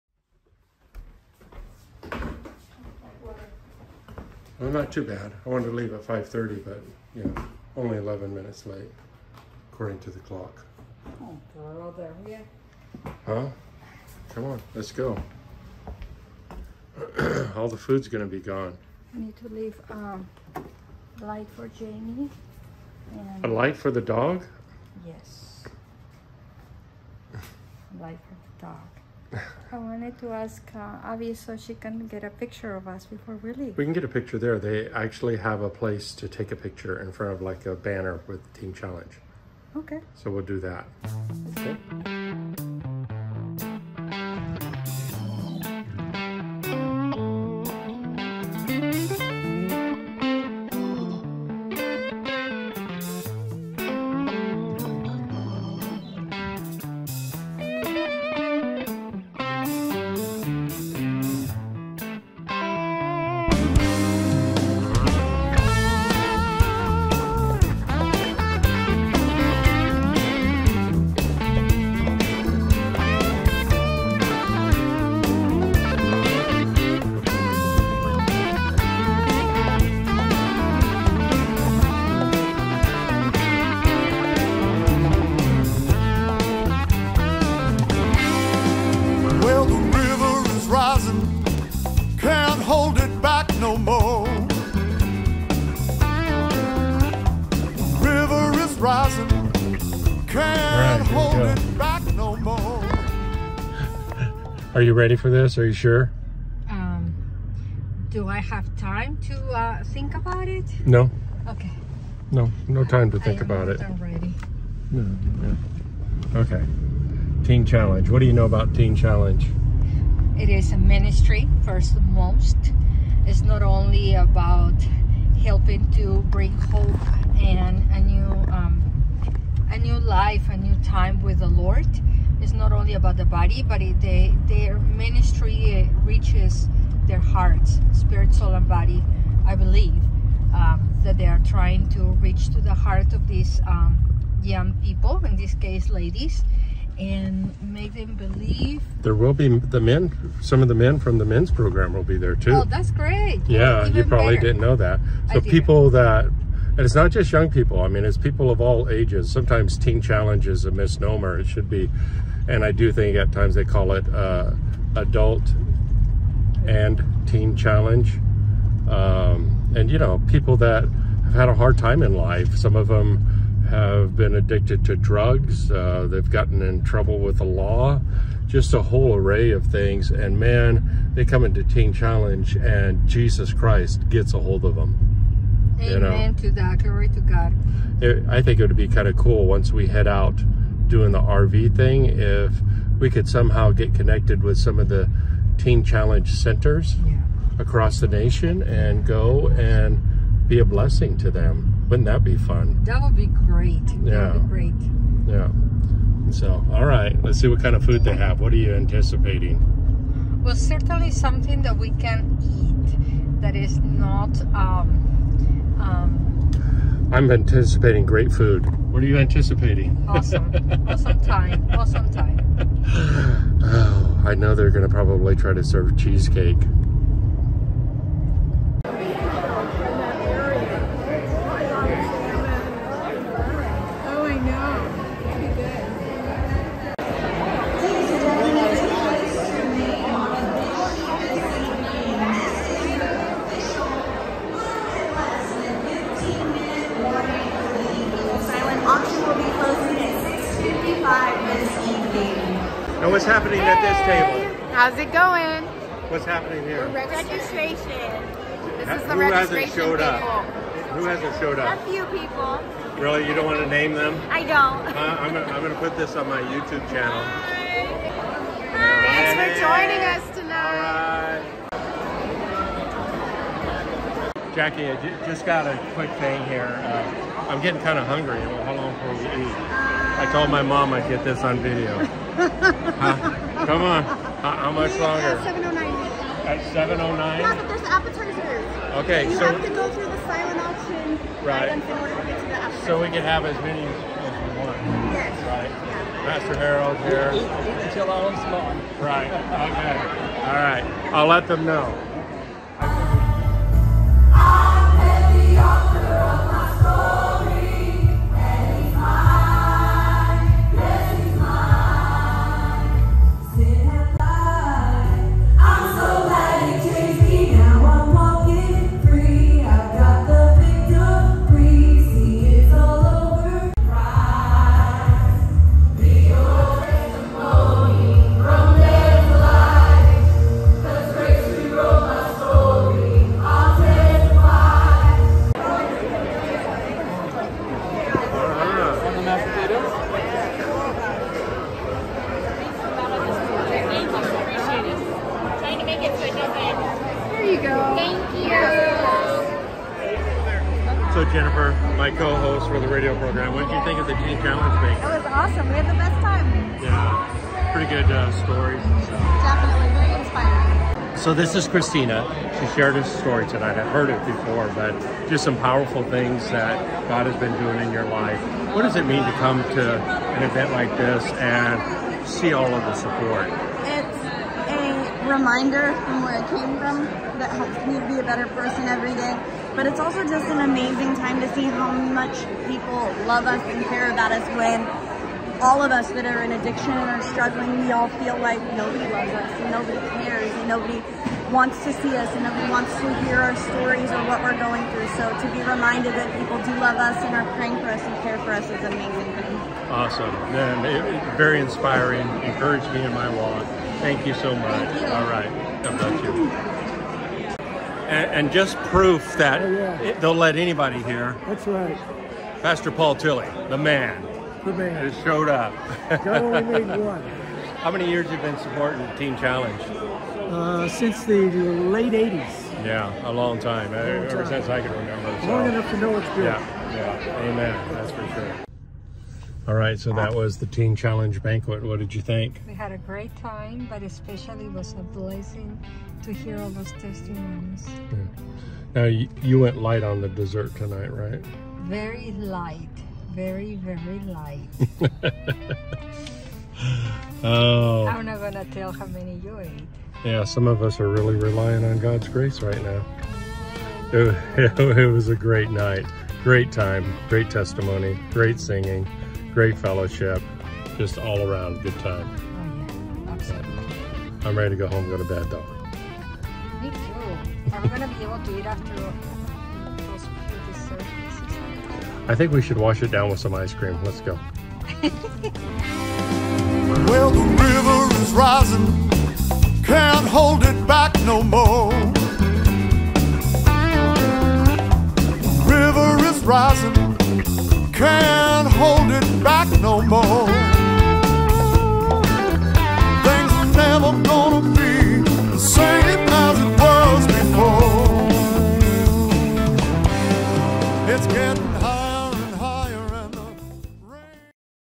Arise Teen Challenge Banquet 2023 | Redding CA
We were blessed to attend the Teen Challenge Arise Banquet this year in Redding California. It was a wonderful evening of songs, testimonies and great food. We were definitely inspired and encouraged in our walk with the Lord.